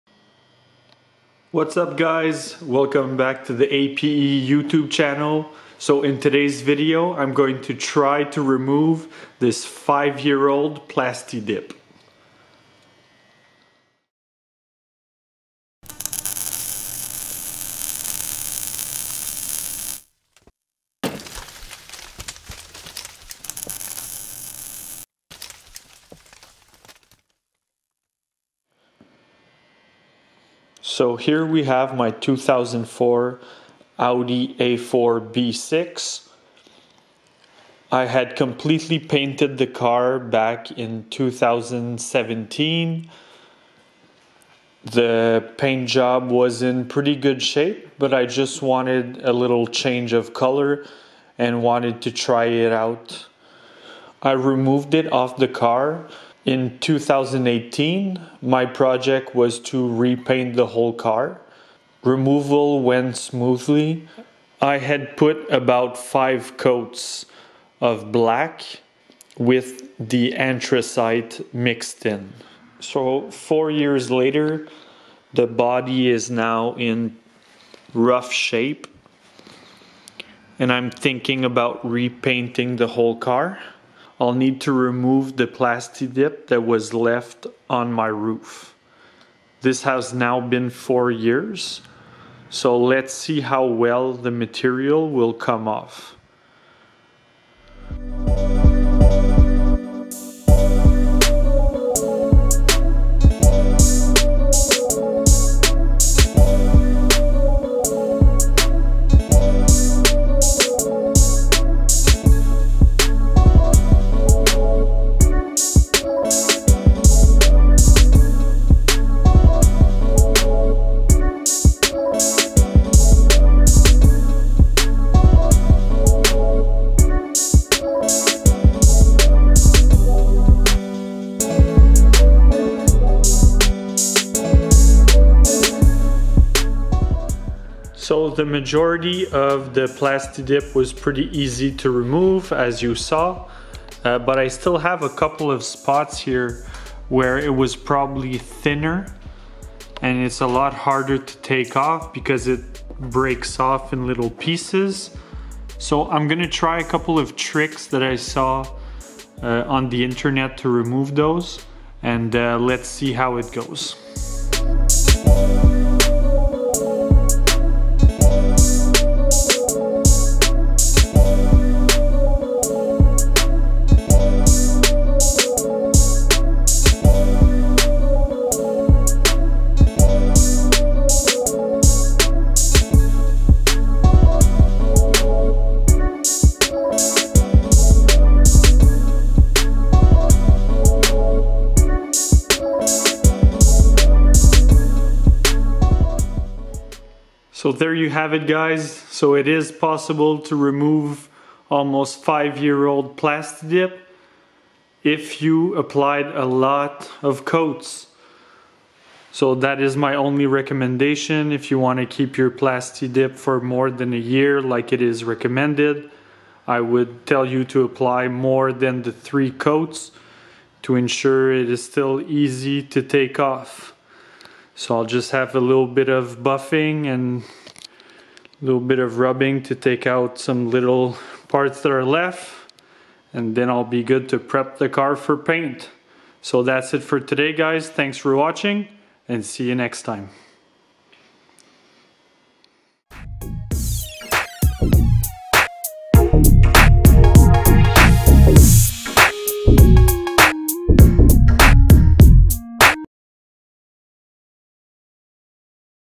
Can you remove 5 YEAR old PLASTIDIP ?
Can you easily remove PLASTIDIP after 5 years ?  I decided to try it out !